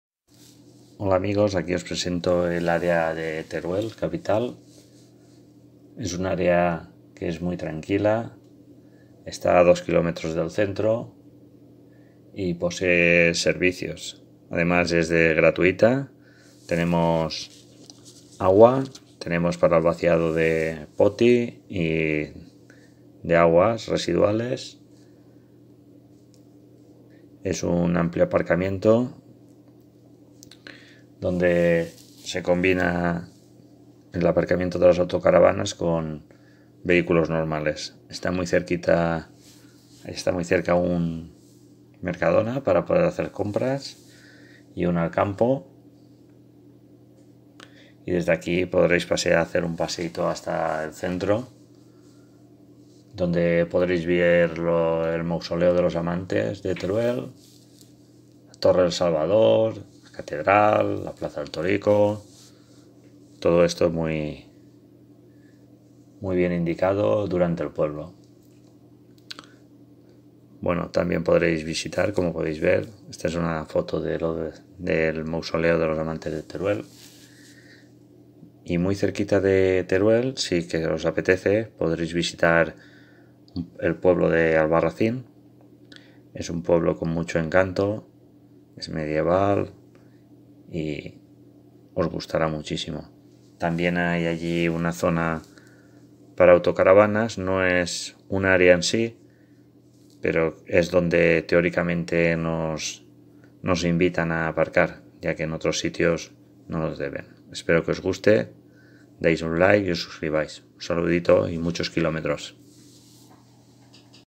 Área autocaravanas Teruel
Área autocaravana de Teruel, gratuita y con servicios. Sitio tranquilo y a 2 km. del centro. Enlace área Teruel Parking Albarracín